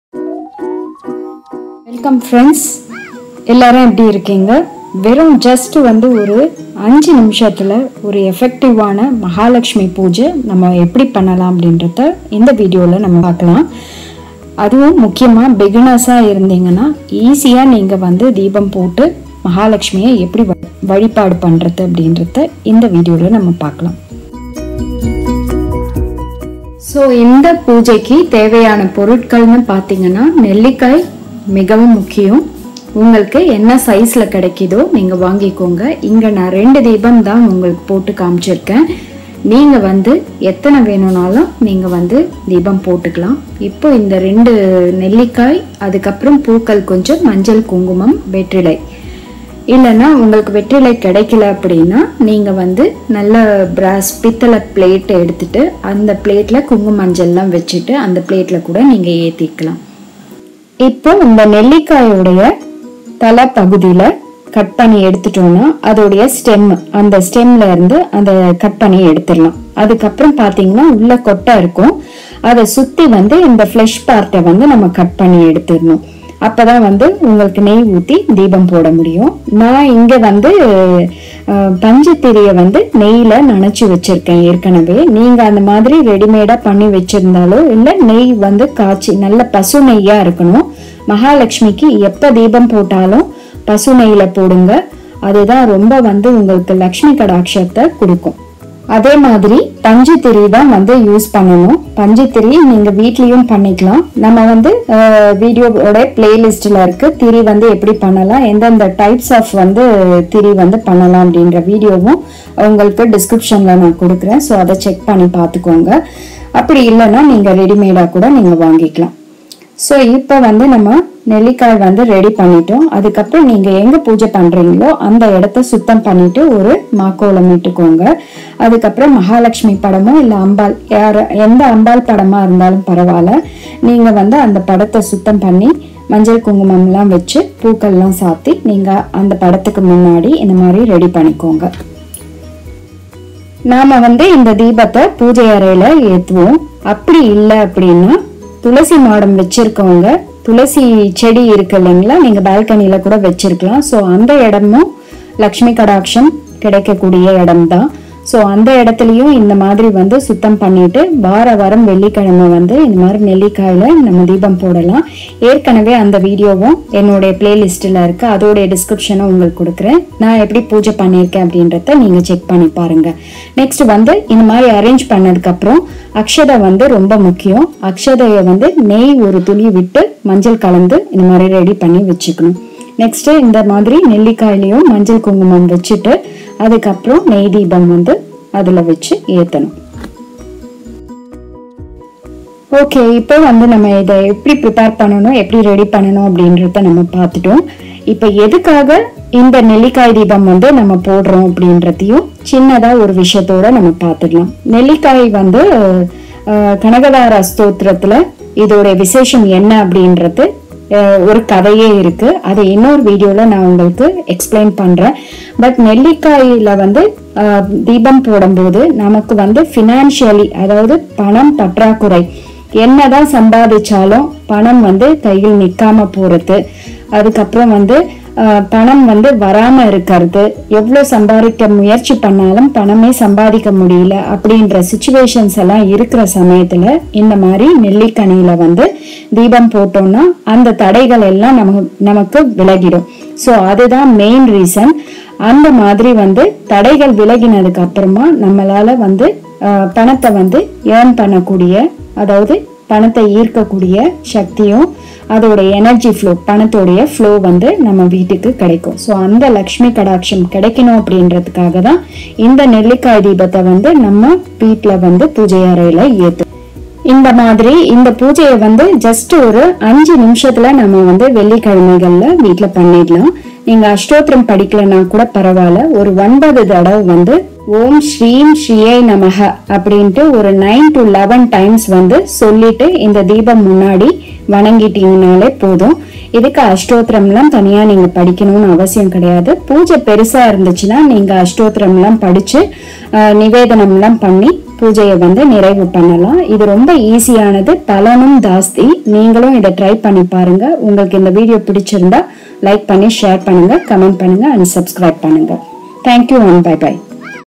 HOW TO LIGHT AMLA DEEPAM AT HOME||FOR BEGINNERS||EASY DEEPAM FOR MAHALAKSHMI ON FRIDAYS..
HOW TO LIGHT AMLA DEEPAM || POOJA & PROCEDURES || DEEPA'S WOW KITCHEN || 5 நிமிடம் போதும், மிகவும் சக்திவாய்ந்த நெல்லிக்காய் தீபம் வழிபாடு செய்ய .